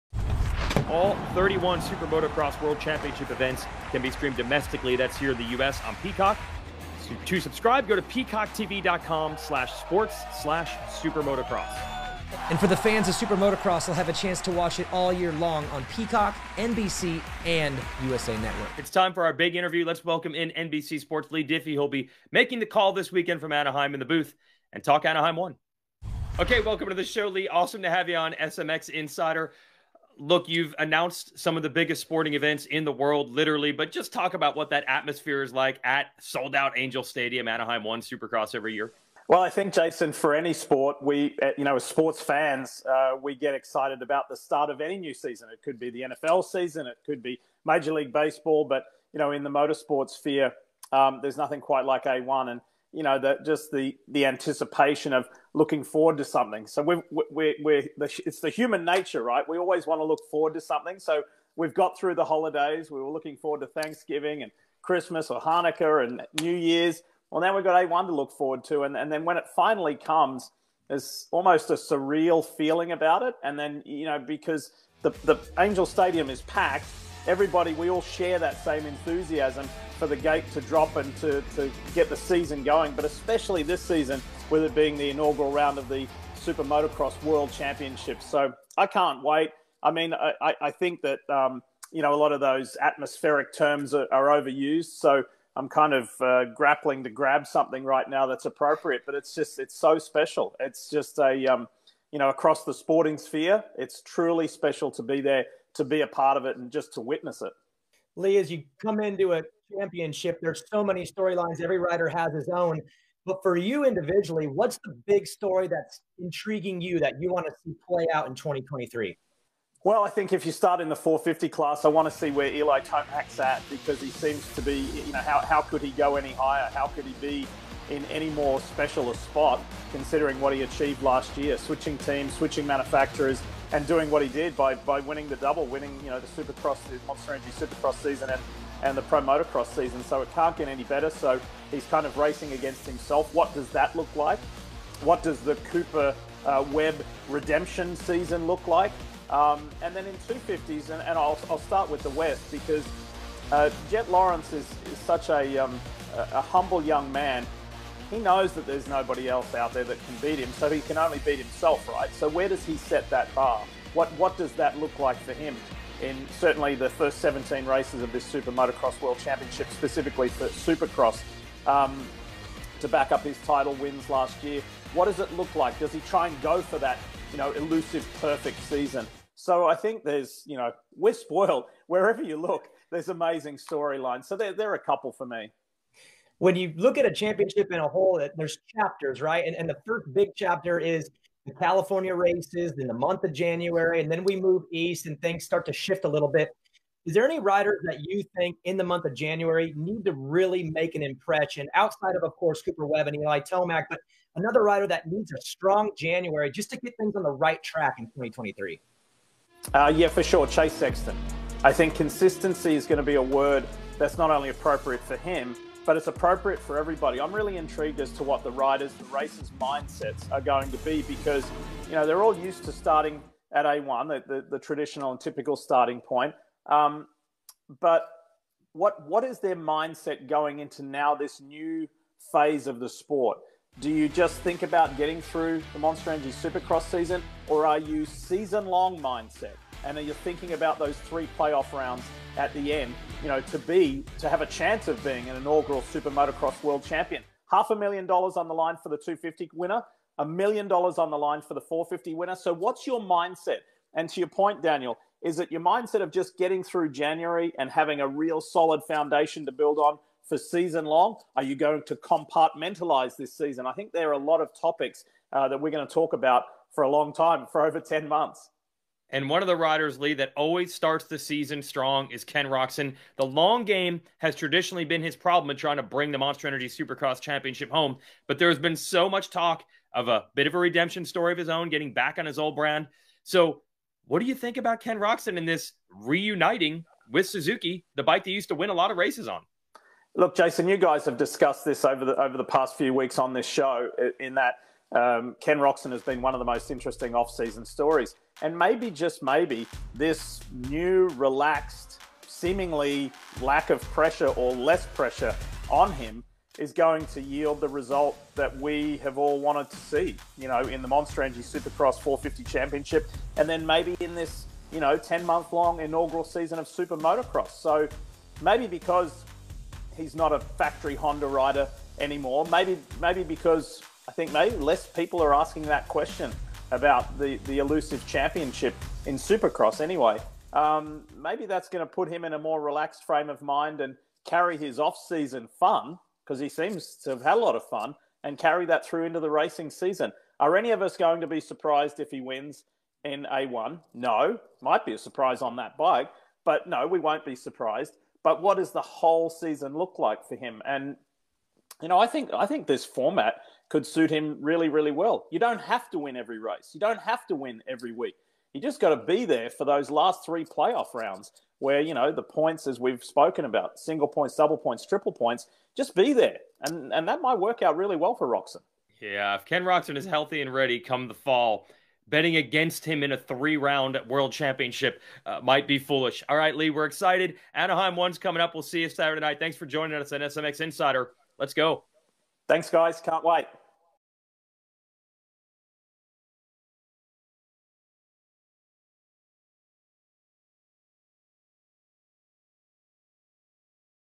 SuperMotocross kicks off with Supercross Round 1 from Anaheim | Motorsports on NBC
Leigh Diffey joins Jason Weigandt and Daniel Blair to preview the Round 1 Supercross event in Anaheim, the top storylines entering the season, outlooks for Ken Roczen, Jett Lawrence, and Chase Sexton, as well as the mindset of riders within a new championship format.